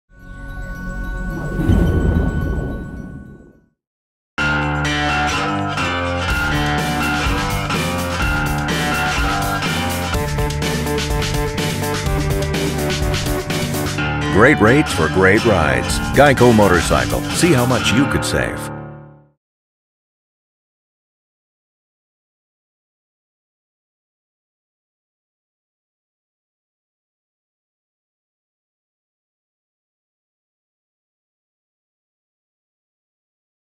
GEICO "Beauty" - DCP Productions
A beautiful ride through the mountains with GEICO Motorcycle